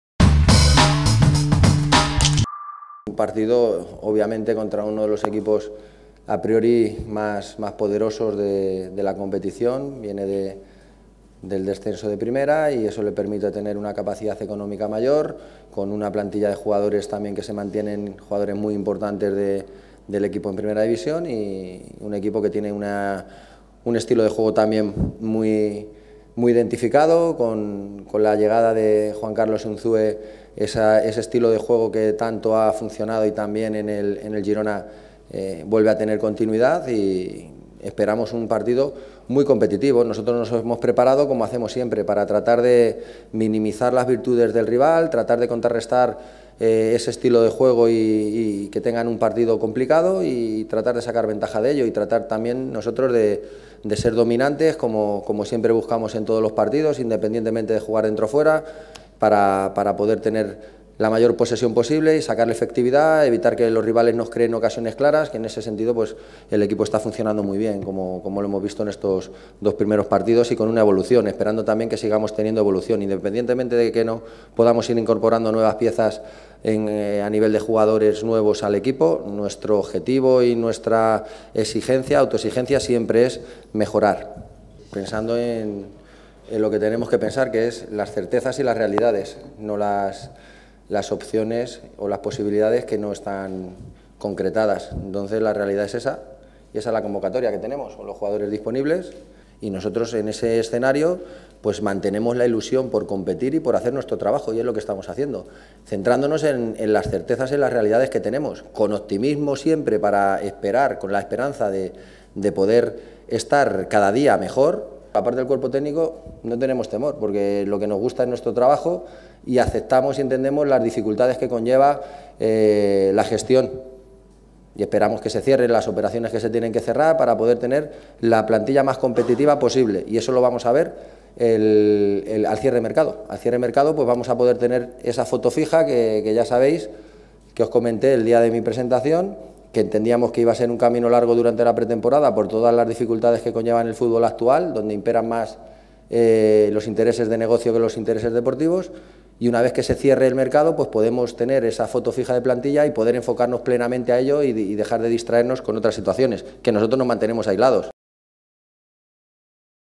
Víctor: “Trataremos de ser dominantes, como siempre buscamos”
El técnico del Málaga CF atendió a los medios en La Rosaleda en la previa del GironaMálaga. “Mantenemos la ilusión por competir con optimismo y esperanza de estar cada día mejor”, resaltó.